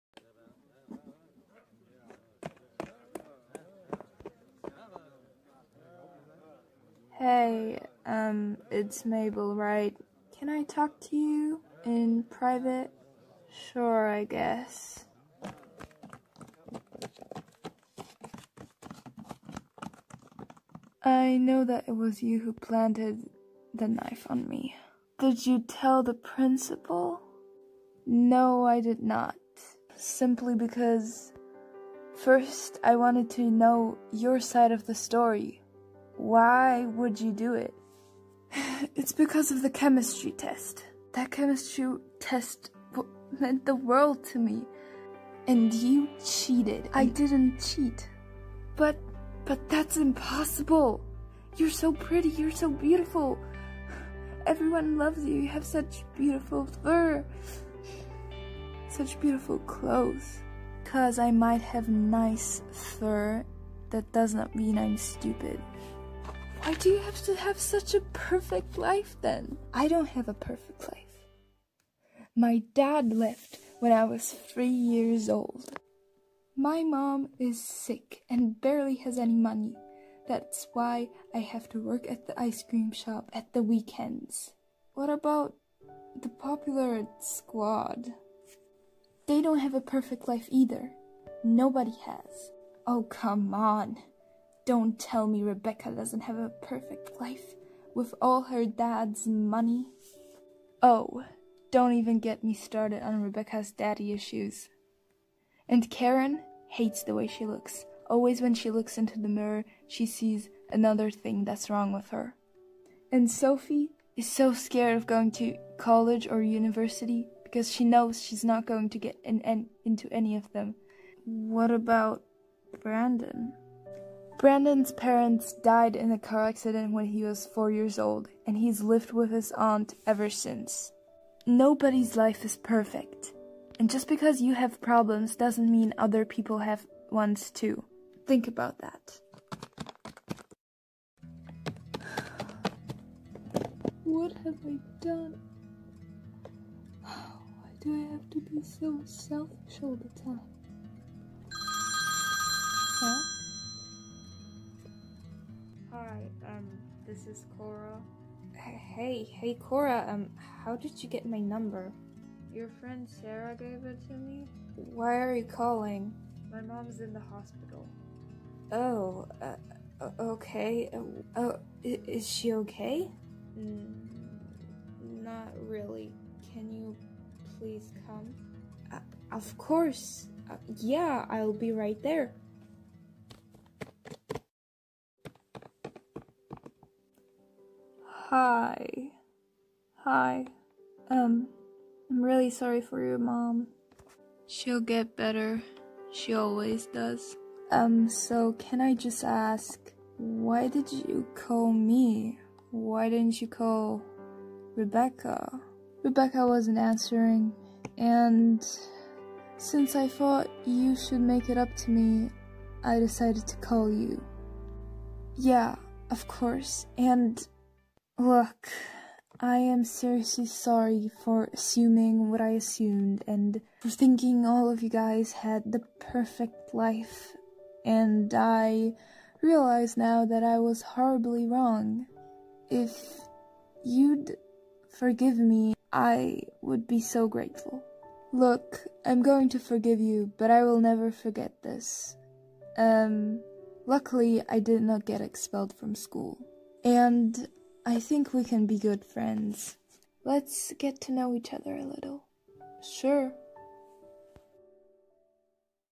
LPS: The perfect life - Part 2/2
Mabel finally learns that not only she has problems and that no one has a perfect life. She decides to make it up to Cora. But how?